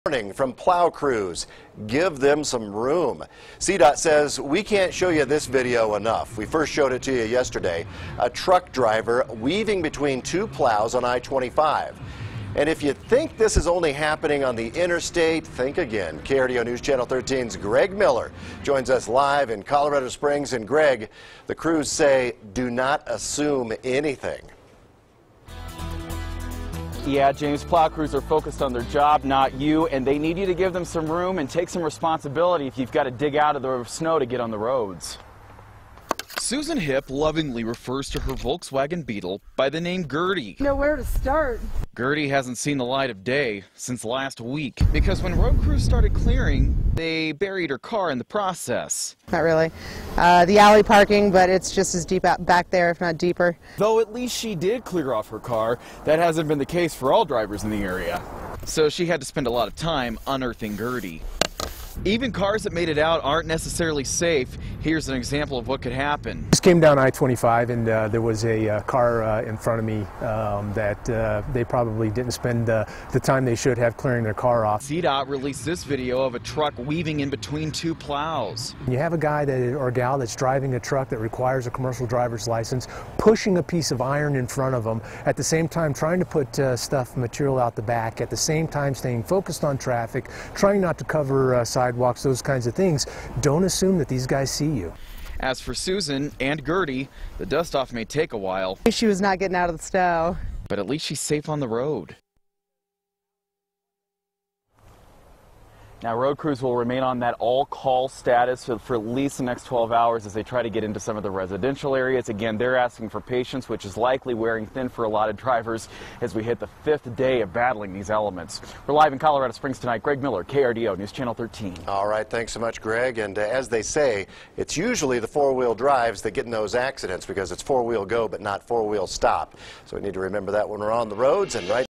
13StormAlert: Crews ask for patience with plow drivers
A warning from plow crews: give them some room. KRDO NewsChannel 13's Greg Miller reports.